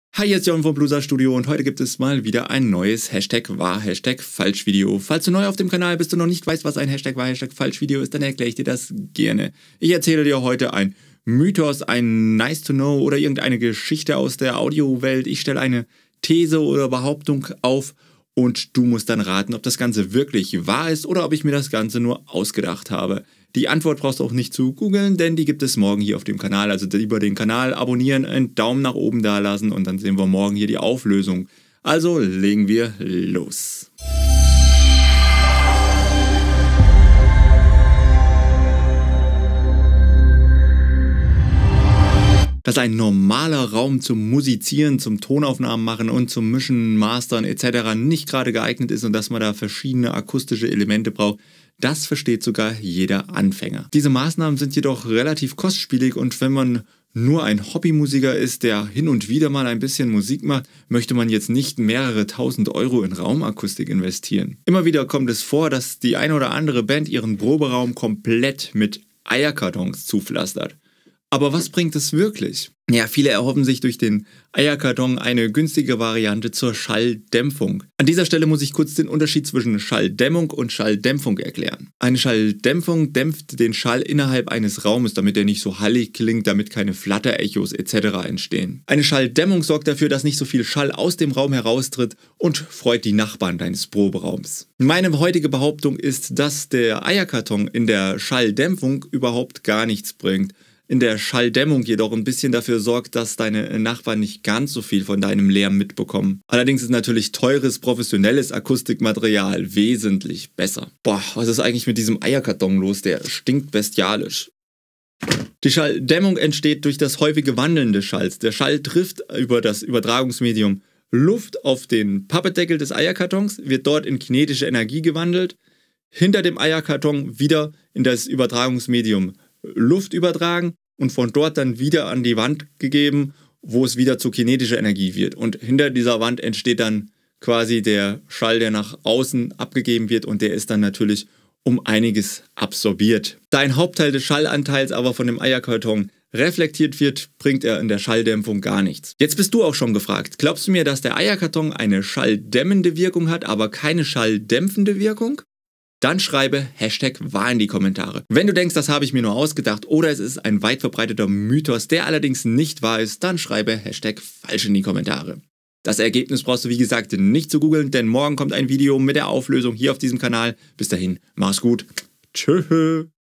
Welche Akustik bringen Eierkartons wirklich? wahr / falsch - Bluesar Studio erklärt
Hast Du auch schon mal Eierkartons an die Wand geklebt, um eine bessere Raumakustik zu bekommen? Günstiger als teure Akustikelemente ist dies aufjedenfall. Aber was bringt diese Methode wirklich? Minimieren Eierkartons den Hall? Oder verursachen sie zumindest eine Schalldämmung / Schalldämpfung?

Dipl. Audio-Engineer und Bachelor of Arts (Hons.) Audio-Production Jochen Gutberlet deckt dies in seinem neuen wahr / falsch Video auf und Du kannst mit raten.

Die Auflösung gibt es hier: cooming soon!

(Tipp: Die Angebote kann man auch ansehen, wenn man kein Facebookprofil besitzt. ;-) )